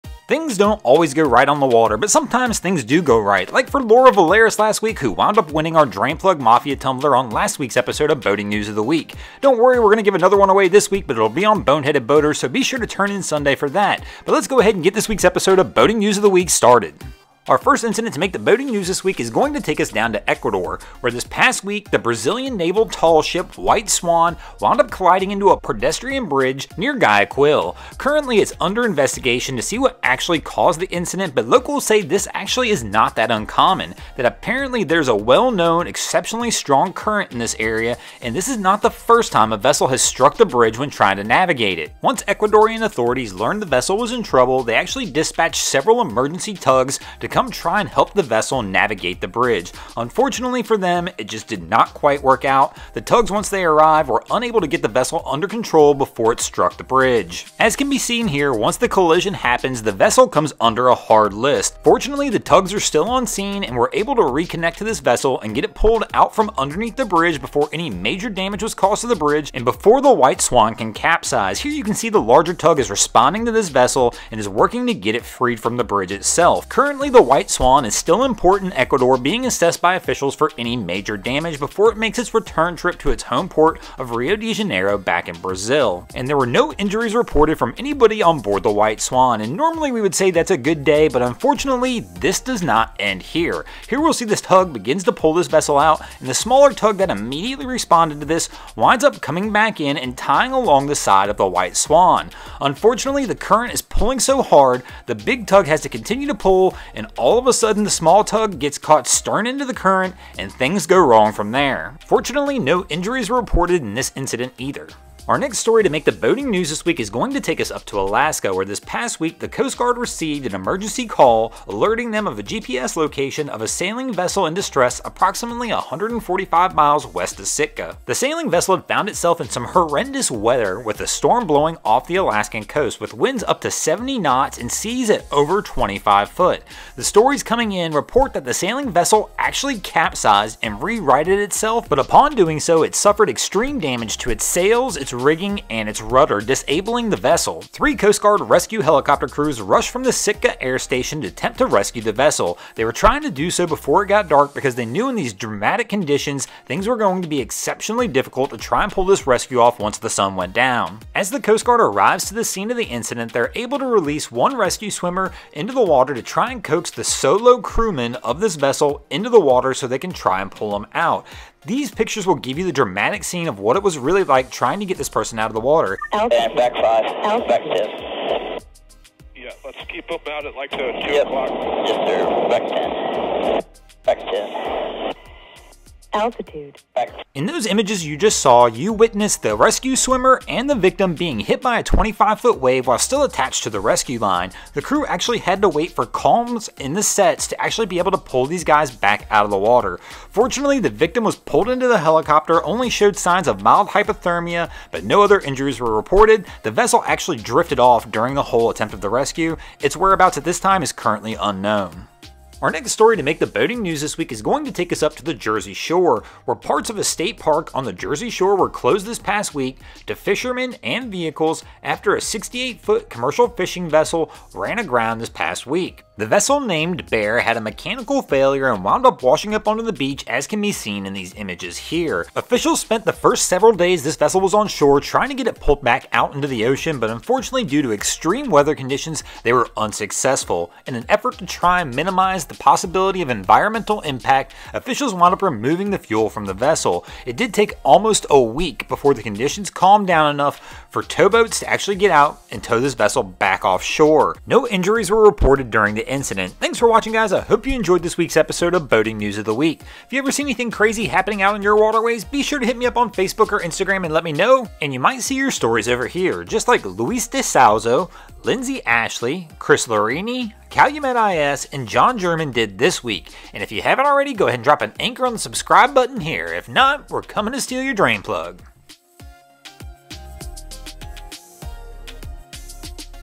Low Bridge | Boating News of the Week
Welcome back to this weeks episode of the boating news of the week where we discuss somethings that made some headlines this week in the news in boating.

  We post this series every Wednesday if you ever see anything you think should make the boating news be sure to reach to us and let us know!!